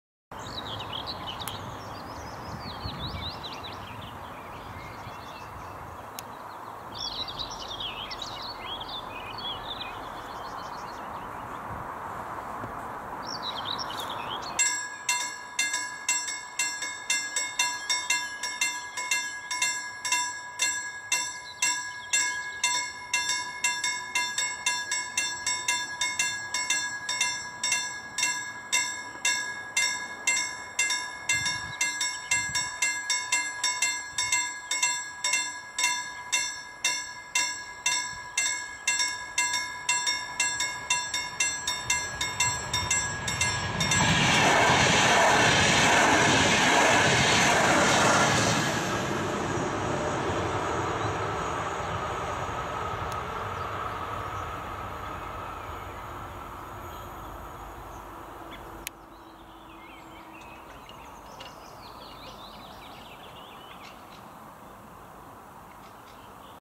InterCity 141 passes Mäkelä level crossing
Finnish InterCity 141 passes Mäkelä level crossing on Partalankoski, Jämsä, Finland.

InterCity 141 ohittaa Mäkelän tasoristeyksen Partalankoskella Jämsässä.